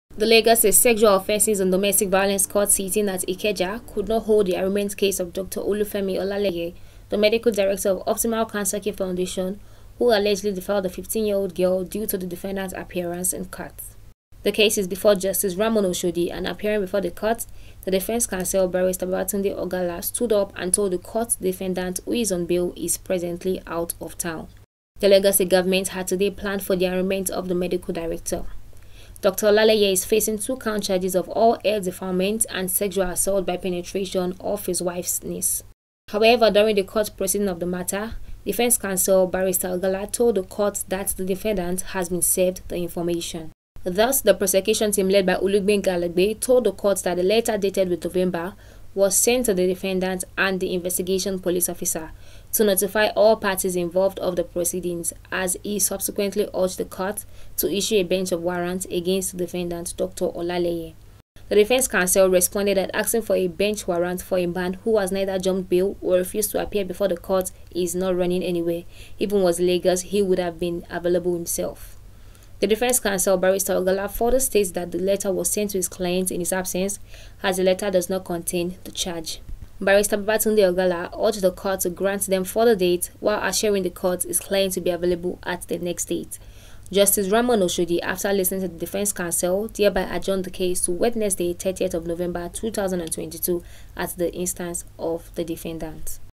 ALLEGED DEFILEMENT
Arraignment Stalls Due To Doctor's Absence In Court